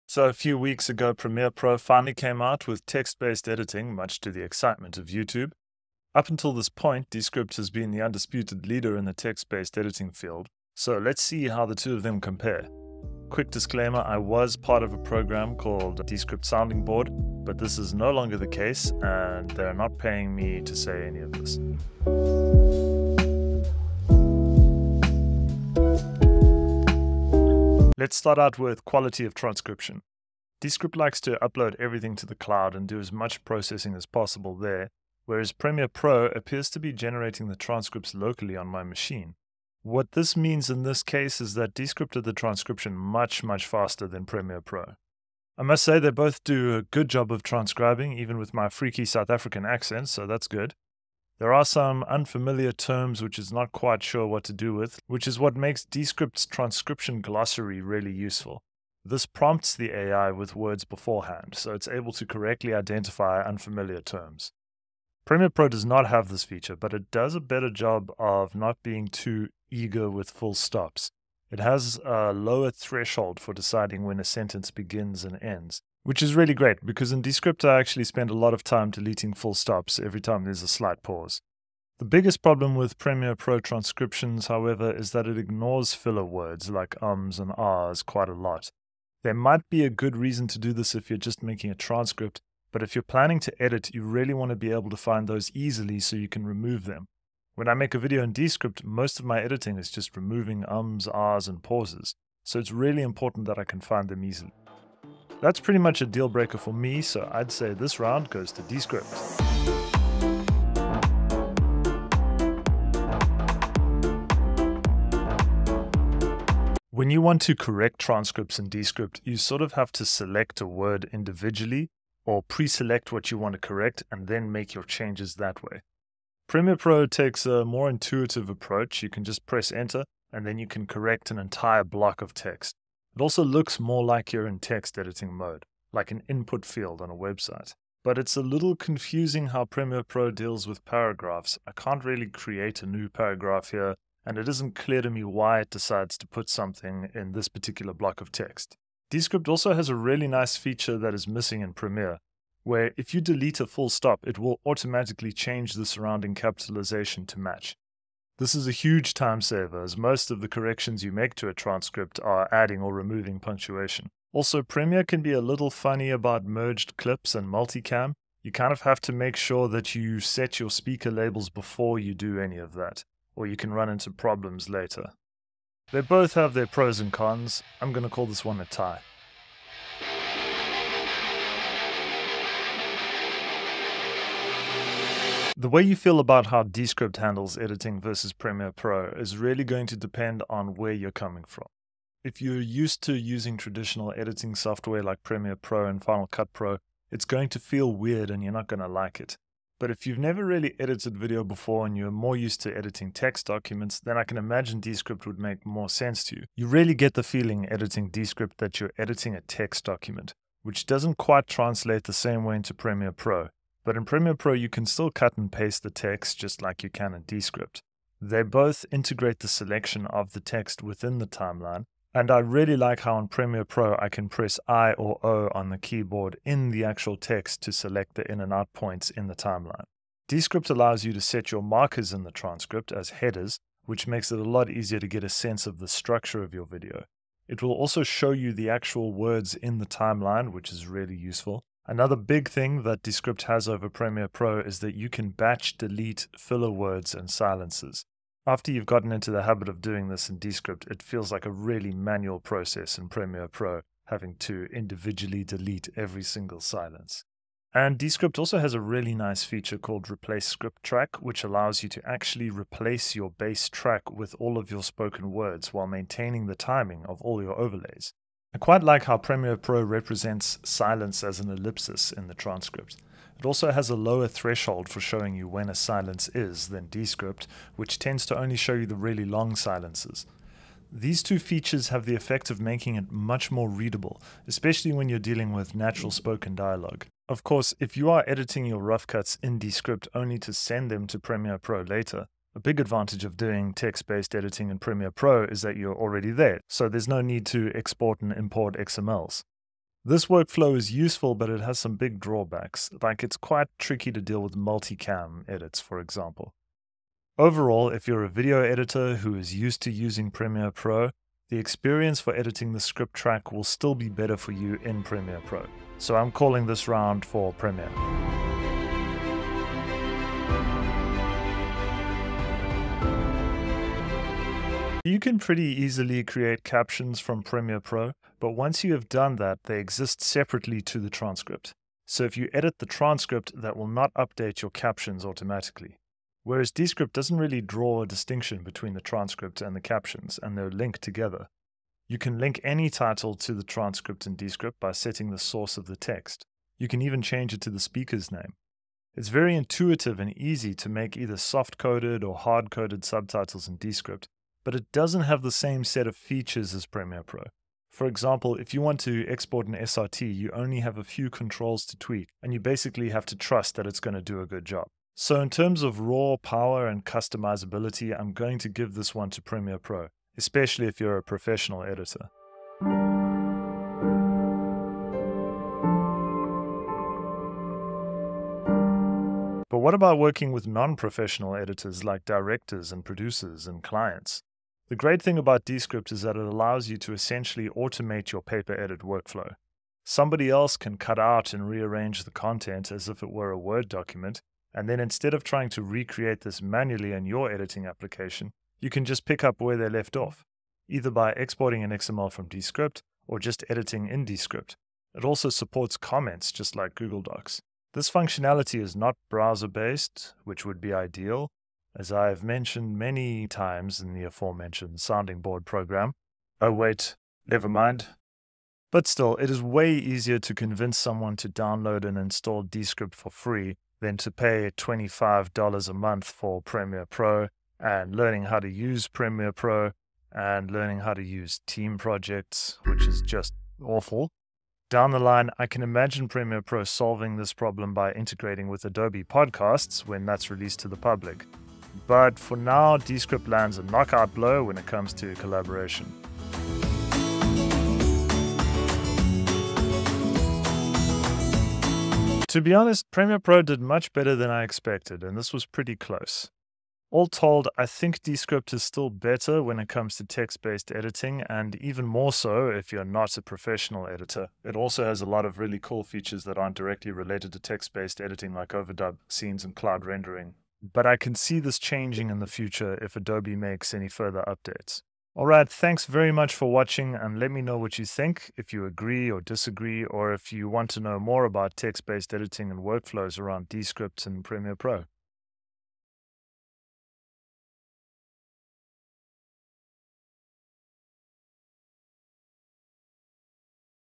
Descript VS Premiere Pro text based editing
Premiere Pro recently incorporated text based editing. Let's see how it compares to Descript.

00:00 Intro
00:21 Quality of transcript
01:49 Correcting transcripts
02:59 Editing
05:17 Captions
06:12 Collaboration
07:26 Verdict